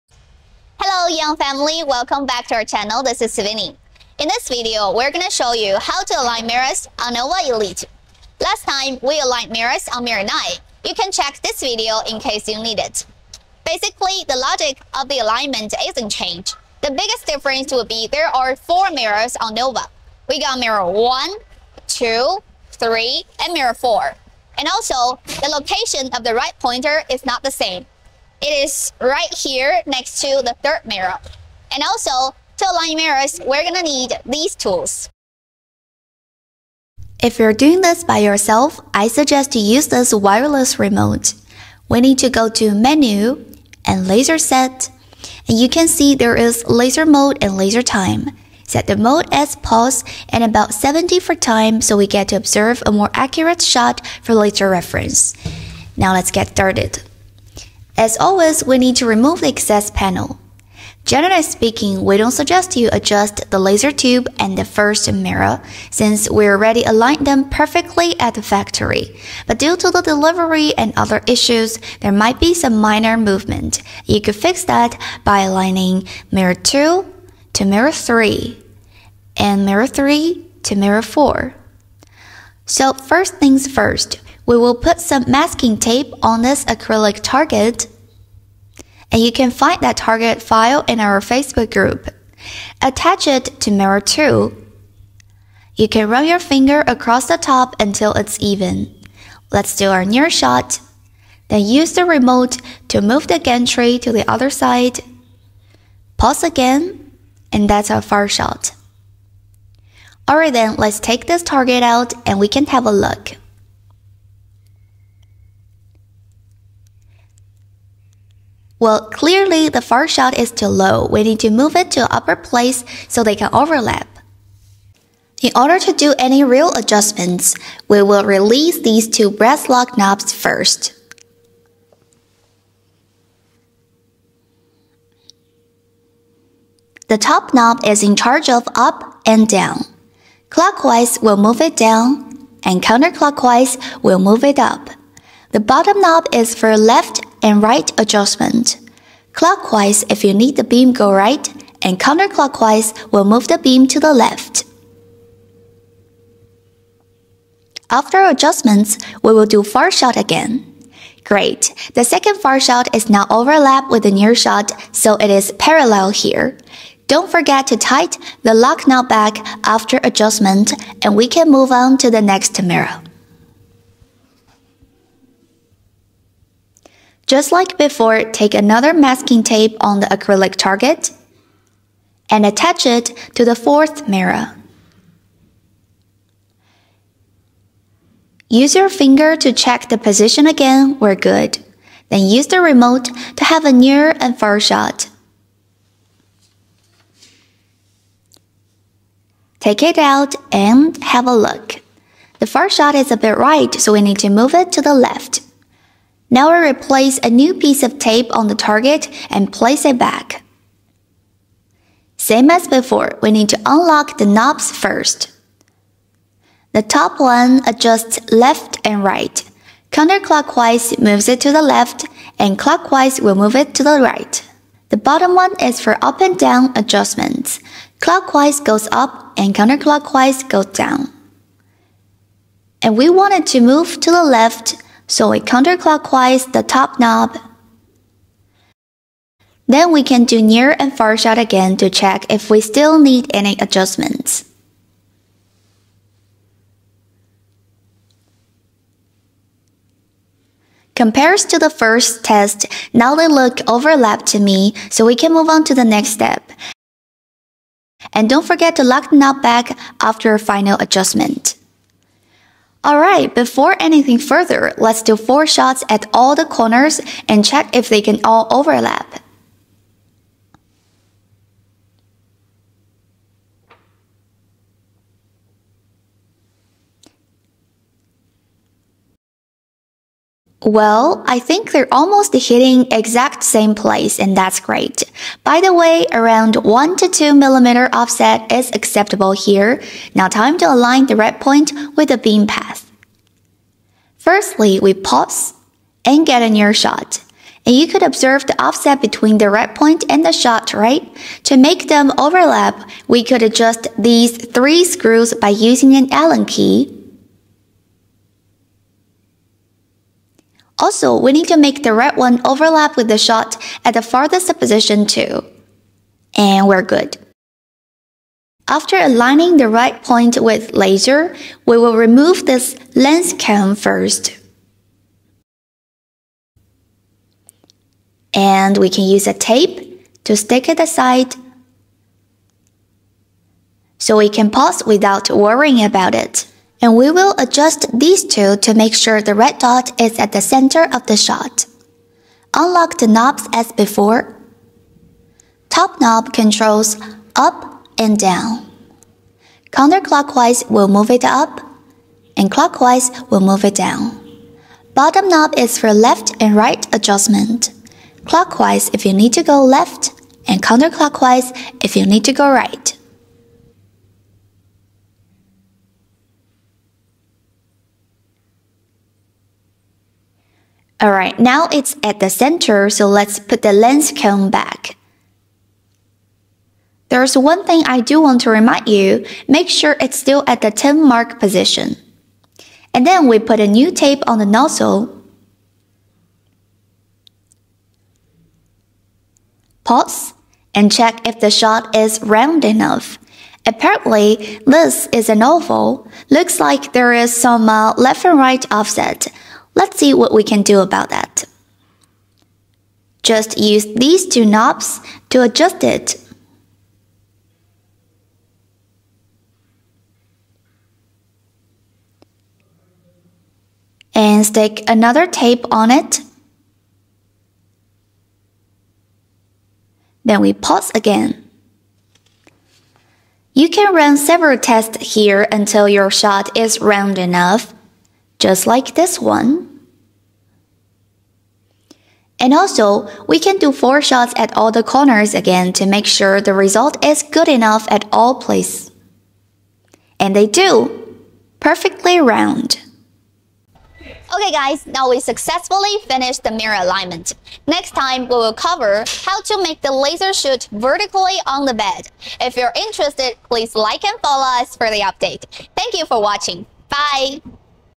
Co2 Laser Mirror Beam Alignment for NOVA Elite | AEON Laser | Tutorial
Hello makers, in this video we will walk you through how to align mirrors on NOVA Elite.
You can find the target file in our FB Group: AEON Laser GLobal.